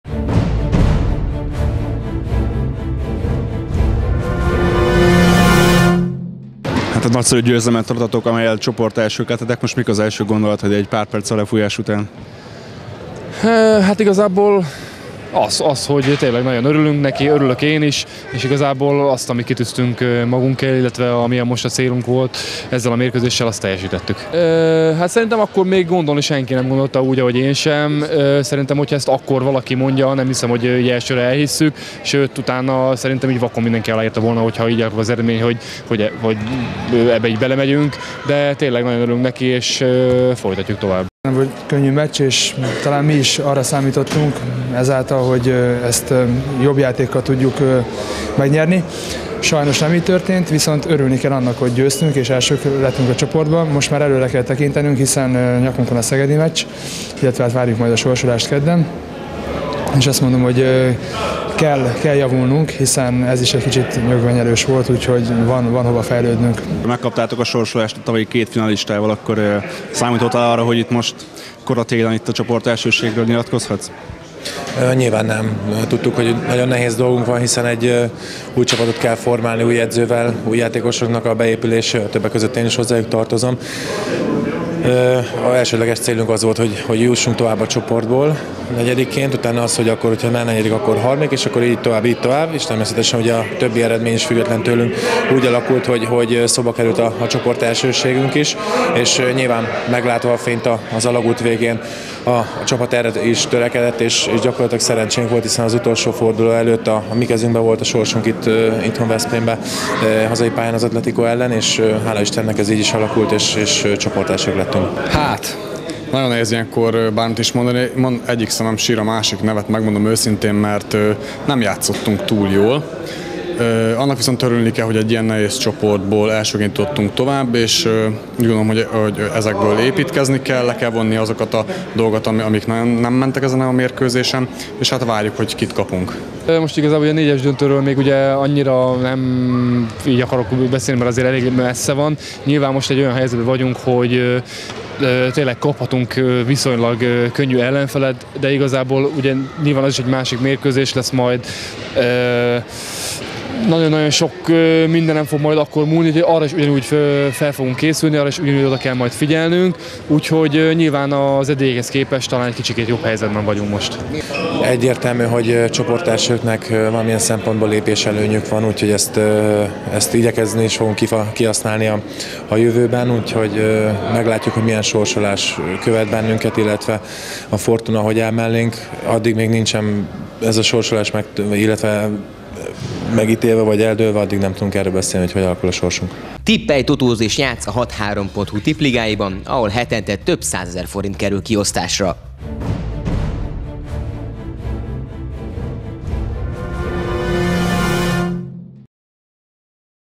Ki kell használnunk, hogy csoportelsők lettünk - Nagy László
Az MKB Veszprém együttese 26-19 arányban legyőzte az Atlético Madrid csapatát a csoportkör utolsó fordulójában, ezzel megnyerte csoportját. A spanyolok felett aratott sikert követően Nagy László, Iváncsik Gergő, Laluska Balázs és Iváncsik Tamás állt a Hathárom Tv kamerája elé.